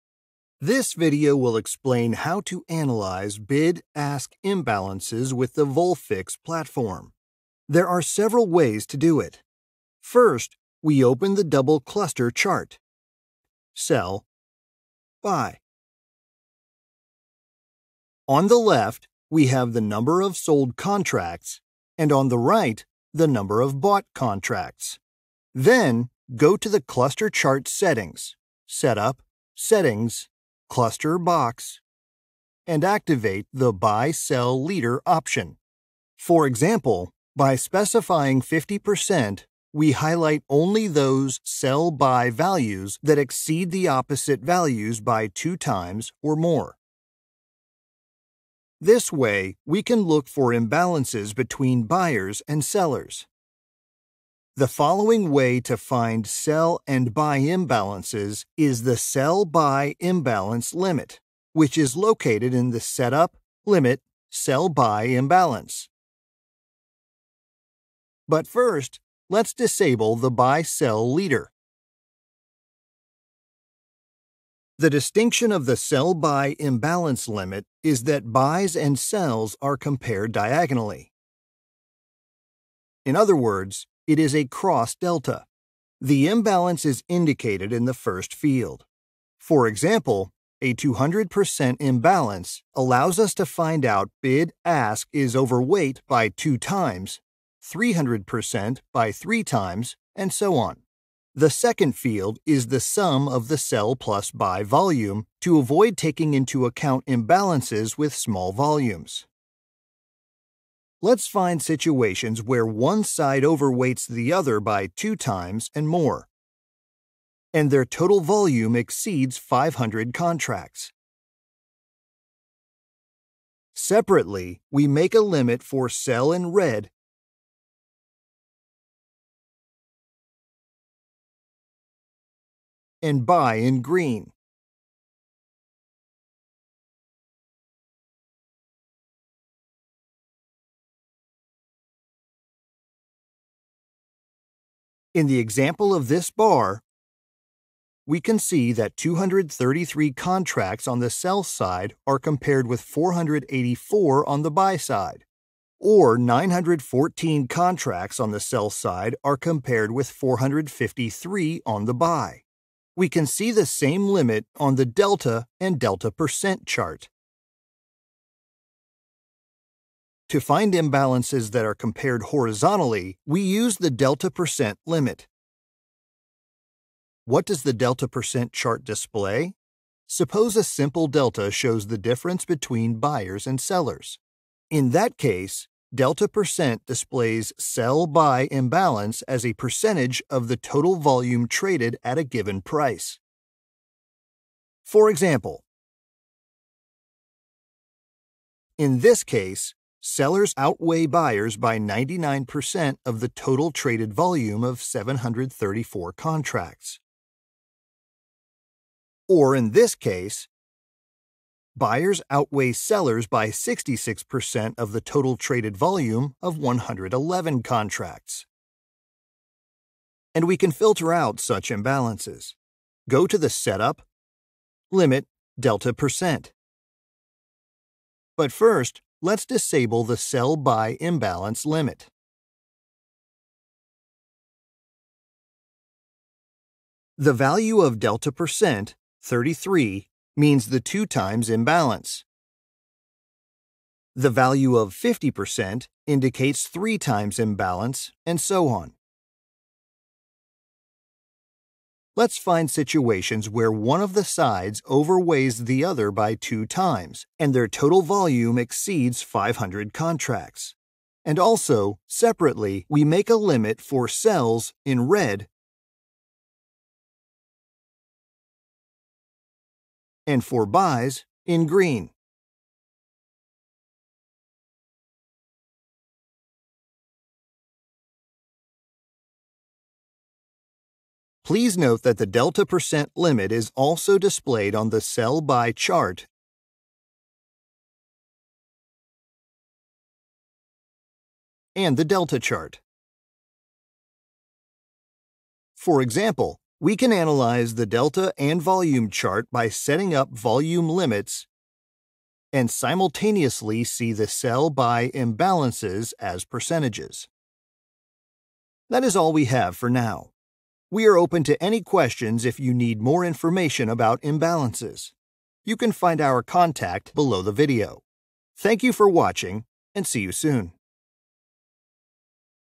Bid Ask Imbalances, Delta%. How to analyze?
00:00 Beginning
00:25 Imbalances search using "Buy-sell leader"
00:54 Sell/Buy Imbalance limit. Cross delta analyzing.
02:50 What is Delta%? Bid/Ask imbalance search with Delta% limit

skype volfix_support